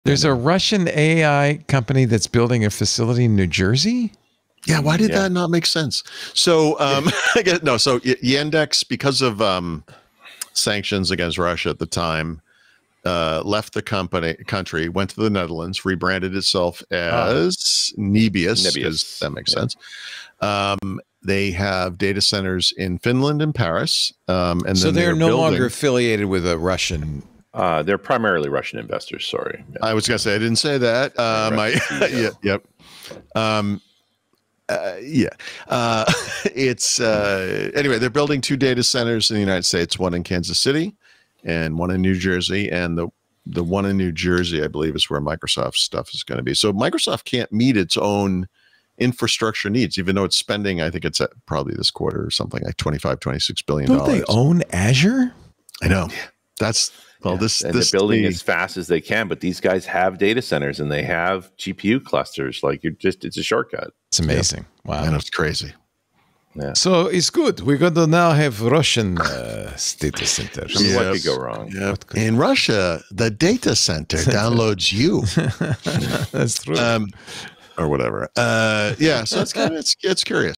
Why Is a Russian AI Company Building Data Centers in New Jersey?!
Wait... there’s a Russian AI company building a massive data center in New Jersey for Microsoft—even though they already own Azure?! 😳 Find out how Yandex (now Nebius) is shaking up the U.S. cloud game, why Microsoft is using their GPU clusters, and the wild story behind it all! What could possibly go wrong?